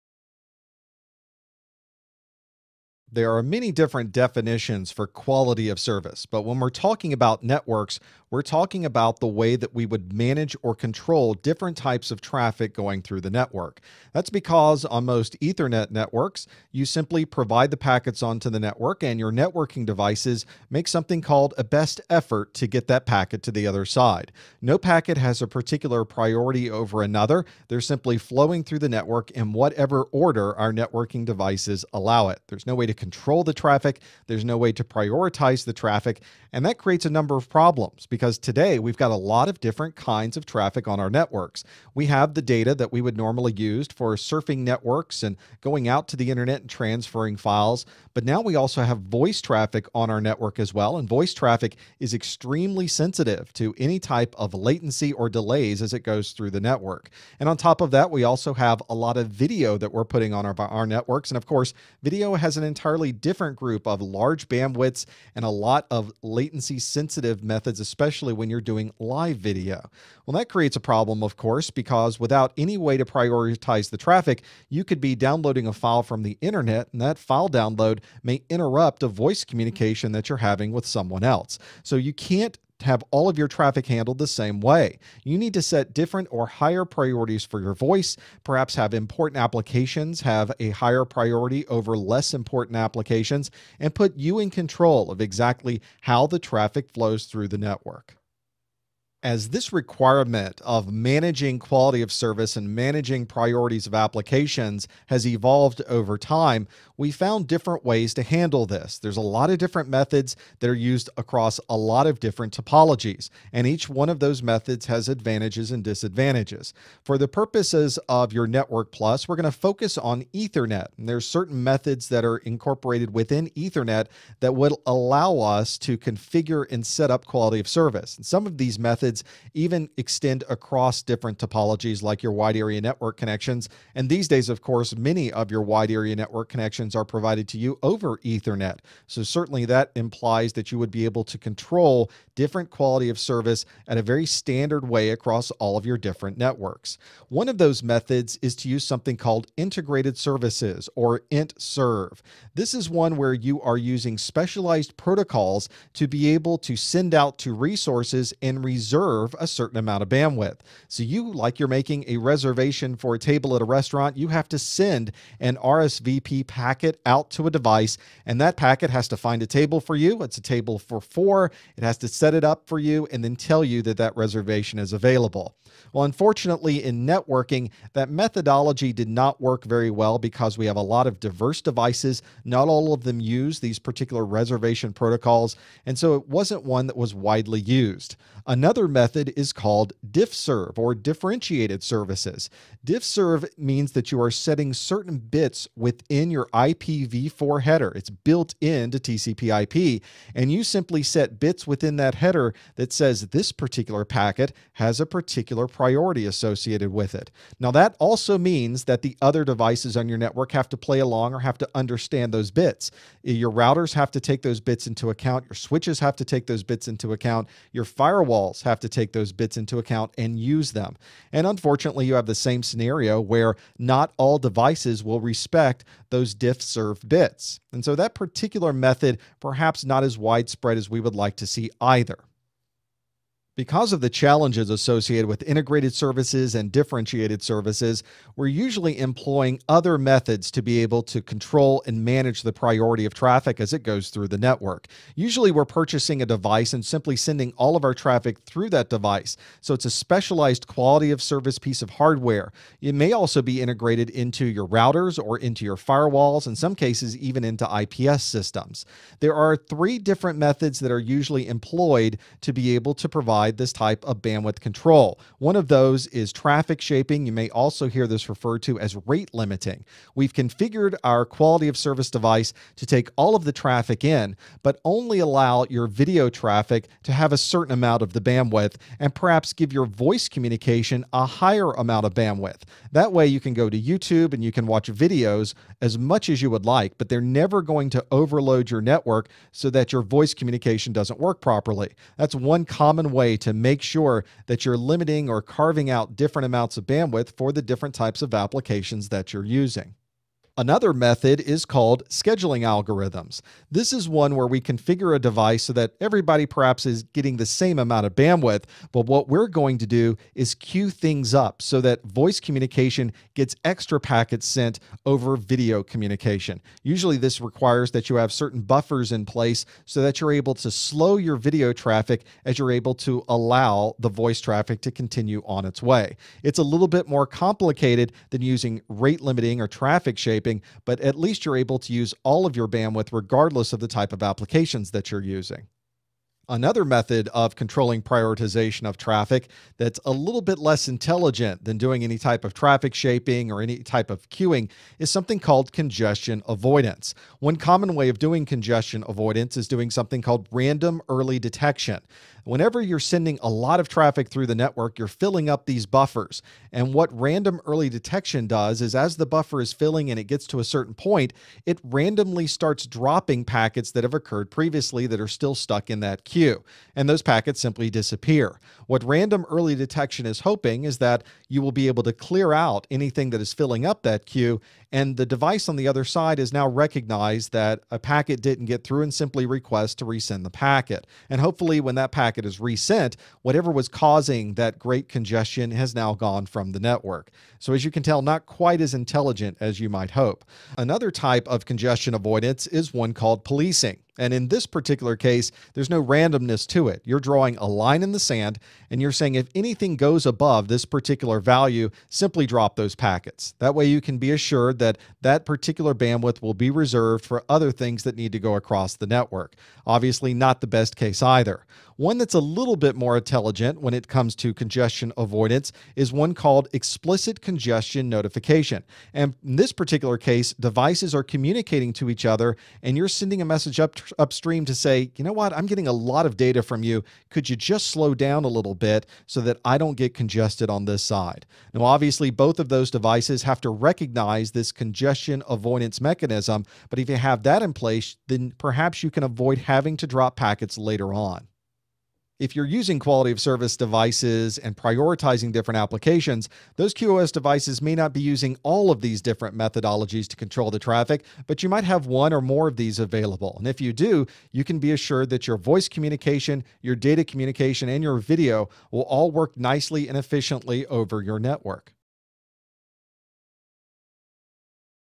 Quality of Service - CompTIA Network+ N10-005: 2.1
With quality of service, you can allow some applications to have a higher priority than others. In this video, you'll learn about many QoS techniques and how you can manage both applications and bandwidth using quality of service.